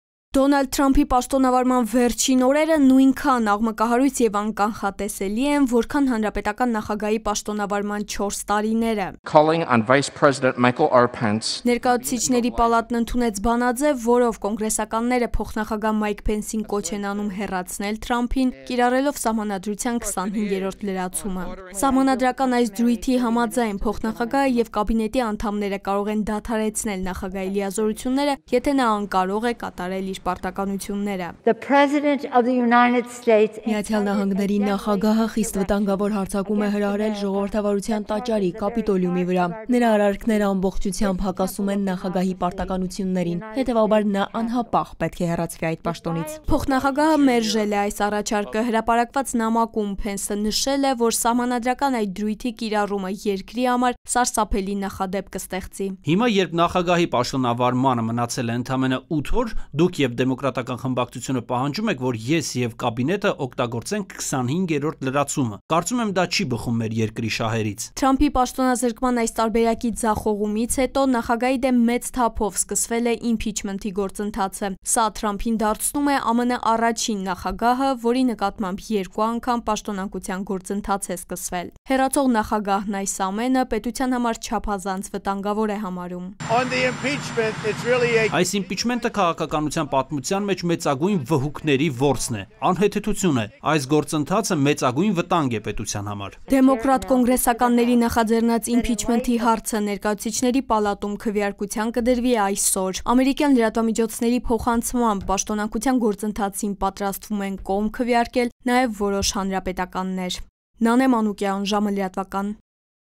ԱՄՆ ներկայացուցիչների պալատը Թրամփի իմփիչմենթը կքվեարկի հունվարի 13-ի երեկոյան
Առնվազն մեկ շաբաթով Թրամփի YouTube ծառայությունը կսառեցնի, վերջերս Թրամփի ալիքում տեղադրված տեսանյութը «բռնություն է հրահրել» և հեռացվել: